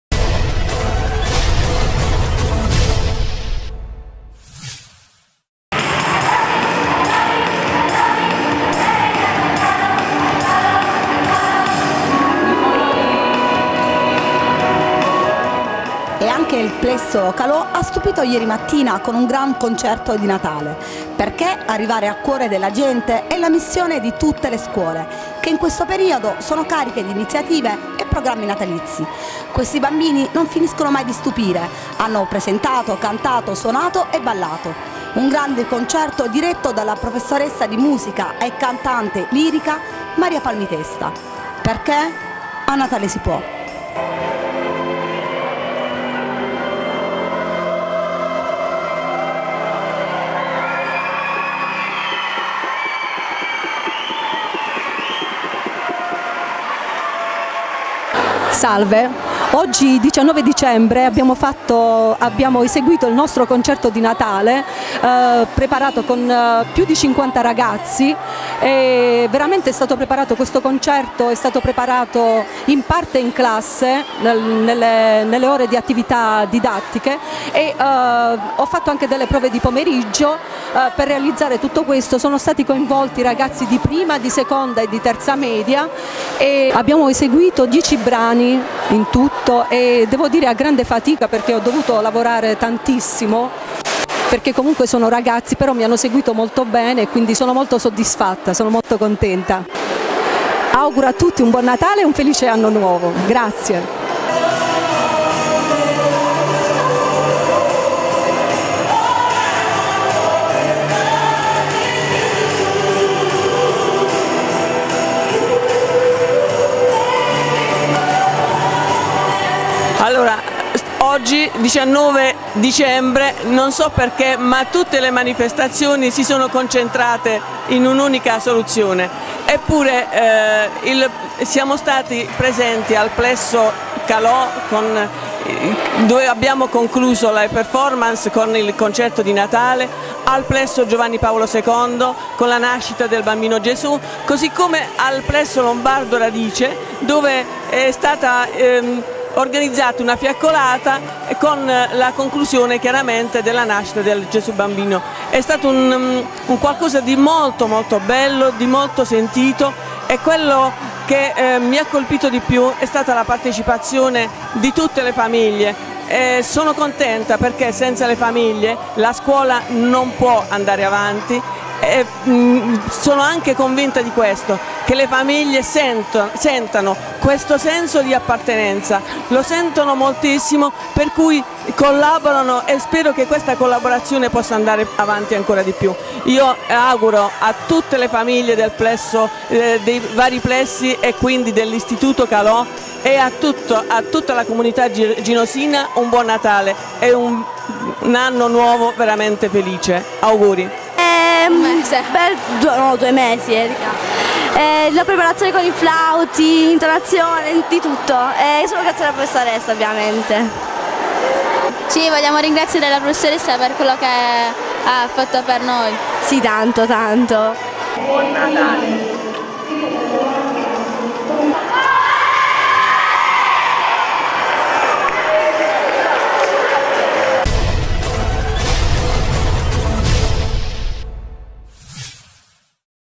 Ginosa: " il Concerto di Natale" Plesso "G.Calò"
I ragazzi della "G.Calò" hanno: cantato, suonato e recitato.